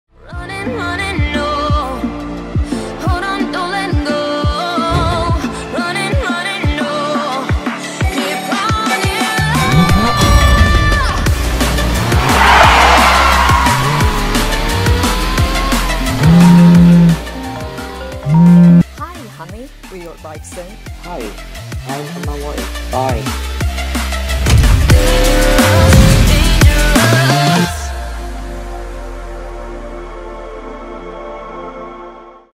【Lamicall】Magnetic Car Phone Mount CV05 - Air Vent Phone Holder Cradle Stand for Vehicle
Lamicall 2021 upgraded magnetic car phone mount features 6 strong magnets, a wider vent grip and a 360° universal ball joint, which can provide enhanced stability and adjustability, bringing you an excellent driving experience.

We're all about helping you to get the most valuable Lamicall electronic accessories！